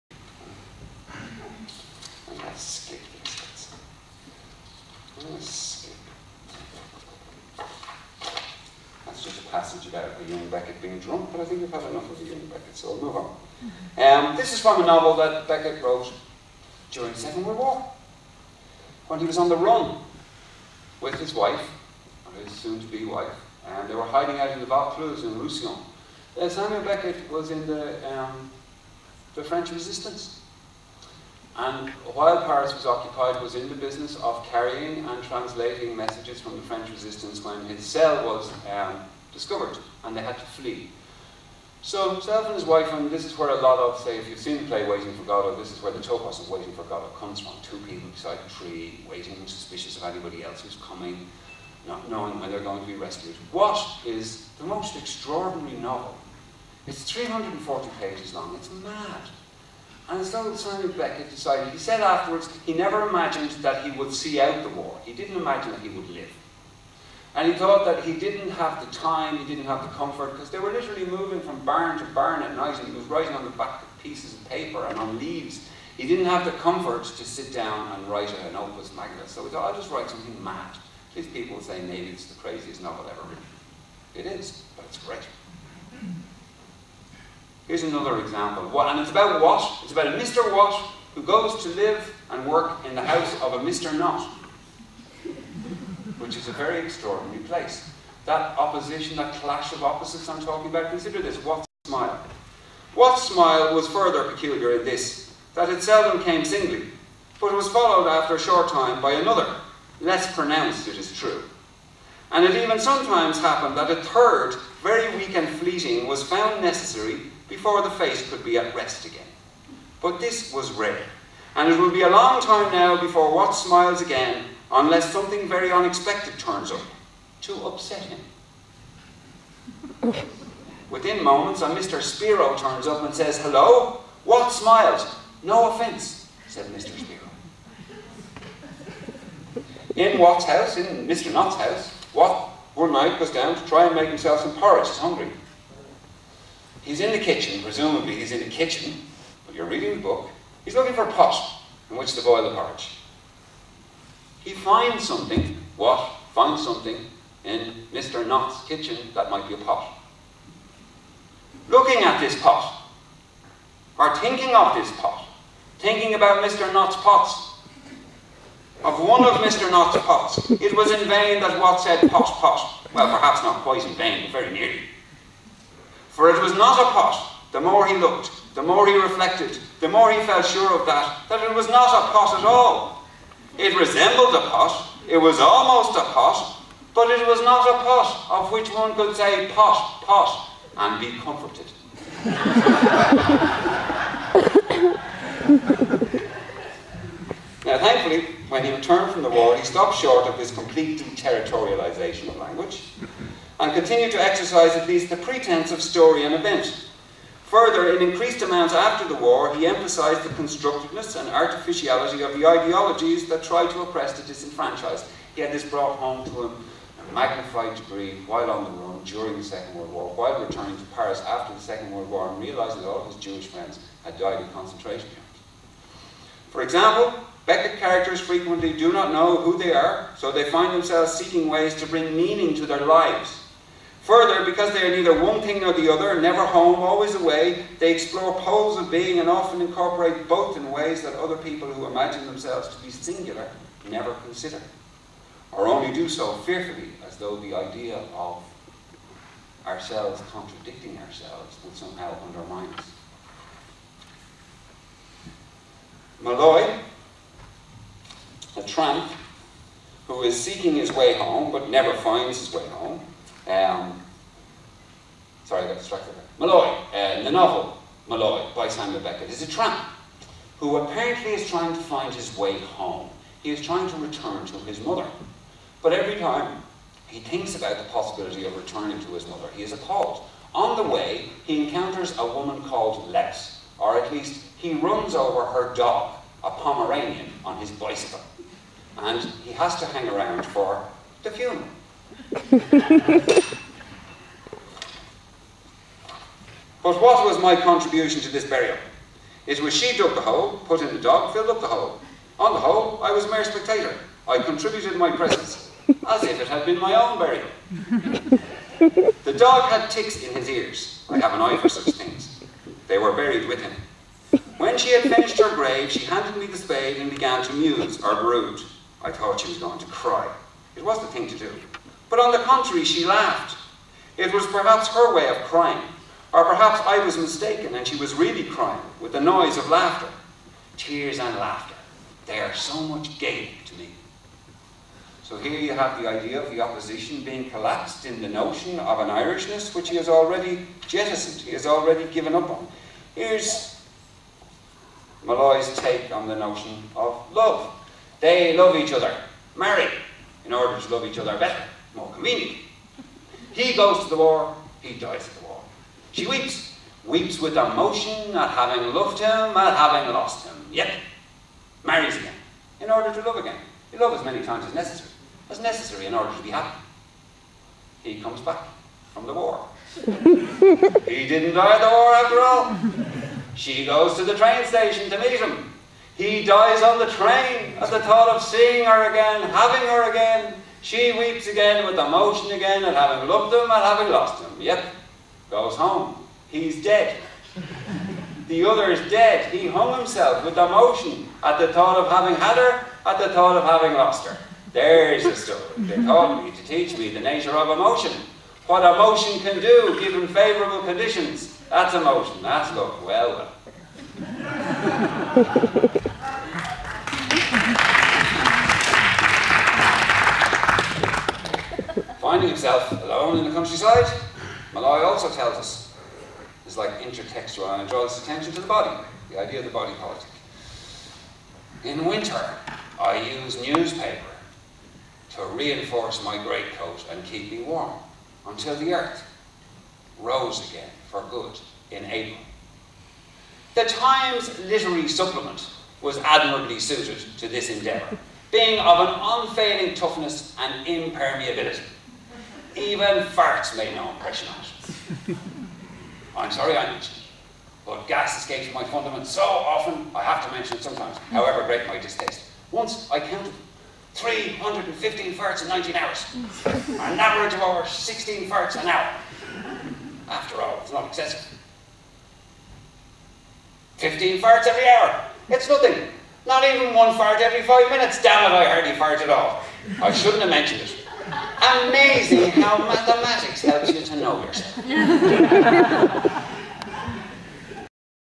Samuel Beckett V.mpeg
'Life In Words' Literary Lecture Series, April 2007. A lecture on Samuel Beckett's sense of humour by Dr. Rodney Sharkey